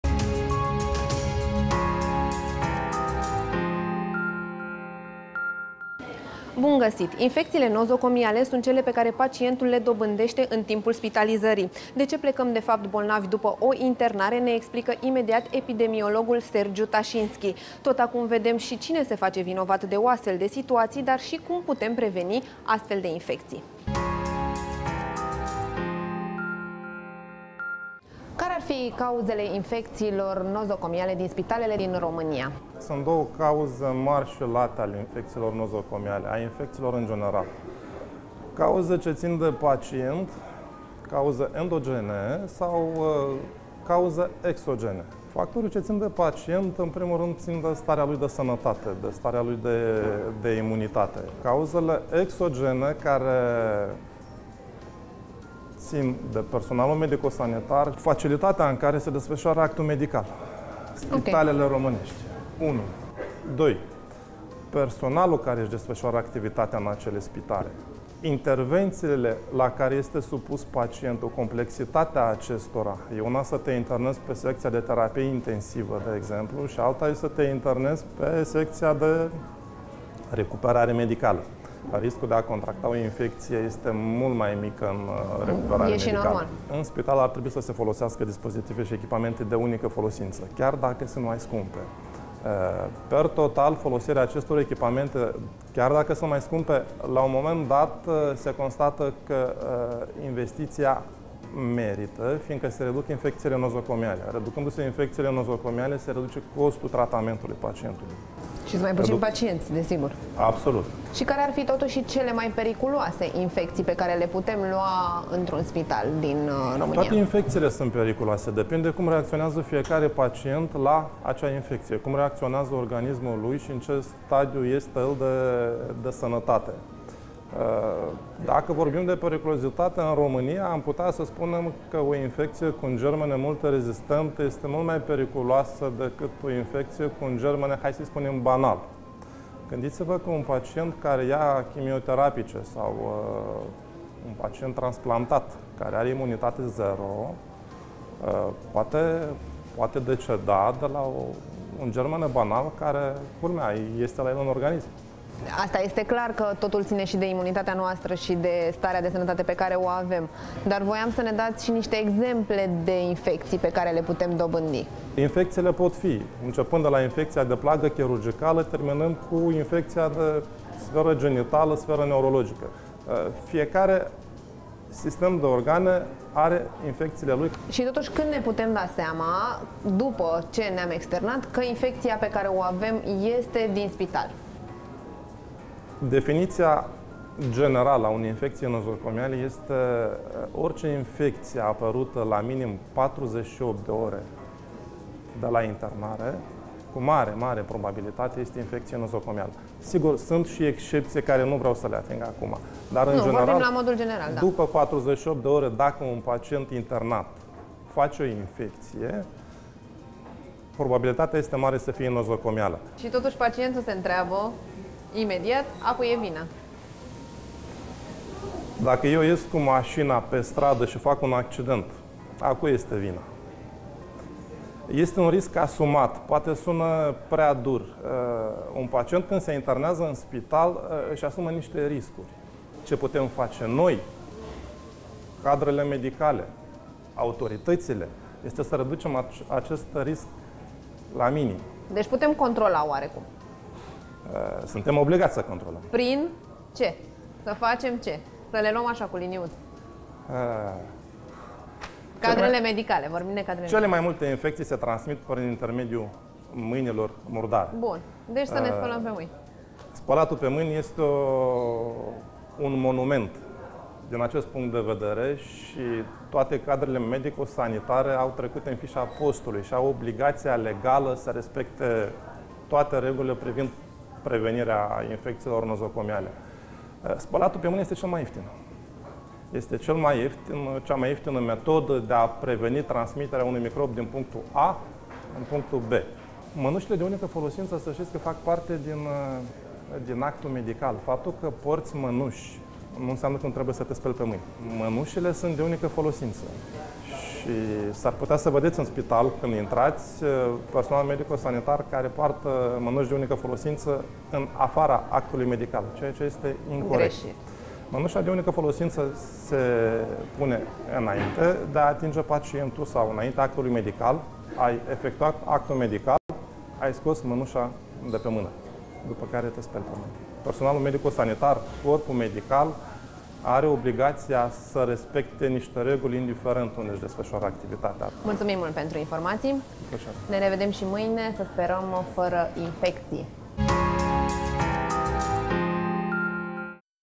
Sfat de Sănătate. Cauzele infecţiilor pe care le luăm din spital
Infecțiile nozocomiale sunt cele pe care pacientul le dobândește în timpul spitalizării. De ce plecăm bolnavi după o internare?

Toate detaliile le-a dat epidemiologul Sergiu Tașinschi, la emisiunea ”Sfat de sănătate”, cu Andreea Cigolea.